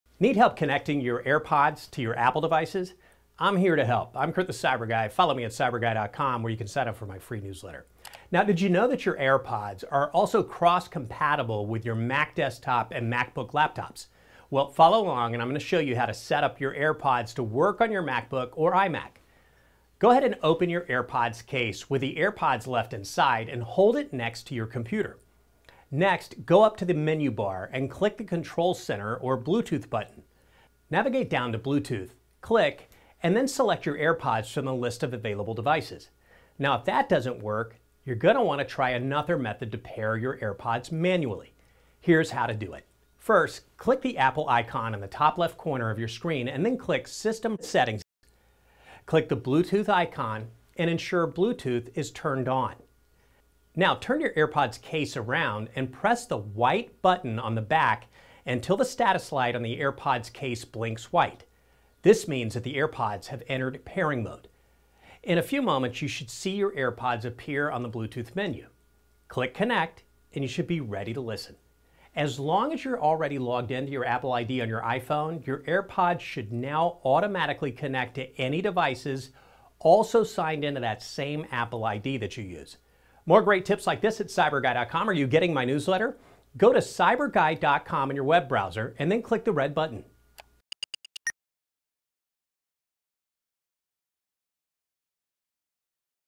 How to connect your AirPods to your Mac and MacBook | Kurt the CyberGuy
Your AirPods are cross-compatible with your Mac desktop and MacBook laptops, providing a seamless listening experience across Apple devices. More:

Cyberguy.com/newsletter

CyberGuy.com/Contact